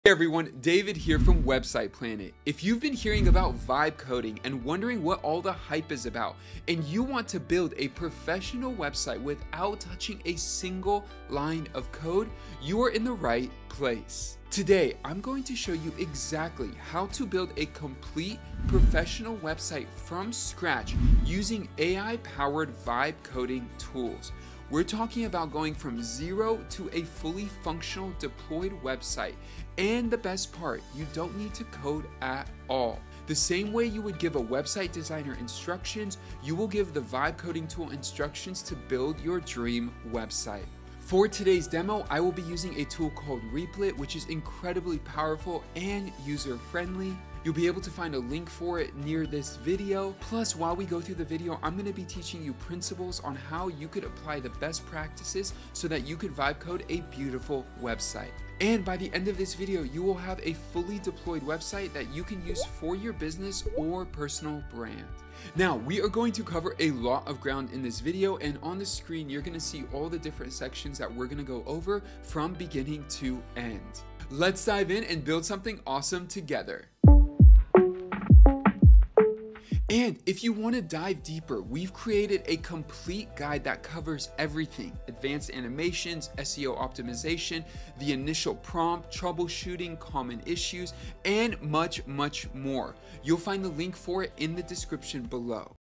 Build a Full Website With AI (No Coding Needed)
Build a complete professional website using AI — no coding required.

This short shows how vibe coding lets you go from zero to a live site by simply giving instructions.
You’ll see how AI-powered tools like Replit handle the design, layout, and code for you.

Links and the full guide are in the description.